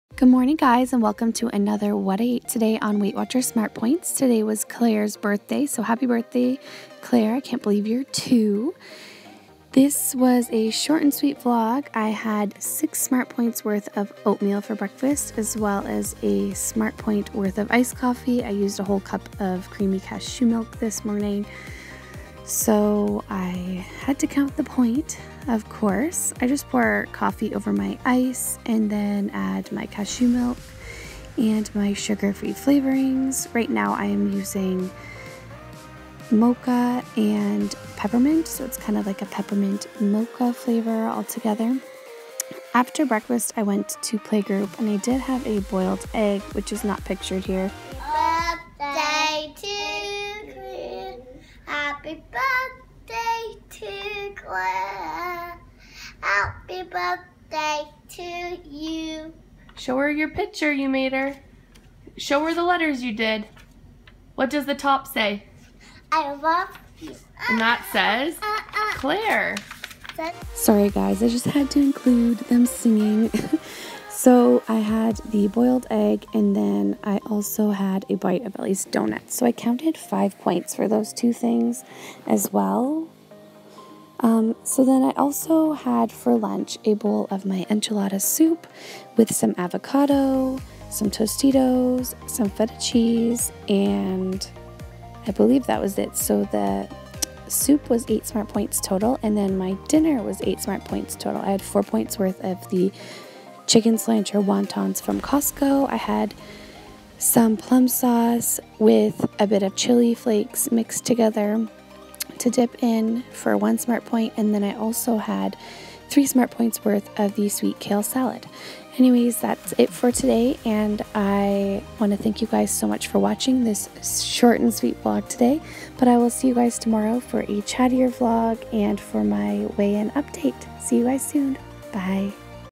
Short & Sweet | What I Ate On 30 WW SMART POINTS
Sorry for the short VLOG with voice over guys! Got quite the migraine this evening and it was the best I could put together 💕

See You tomorrow for my week 5 maintenance weigh in !!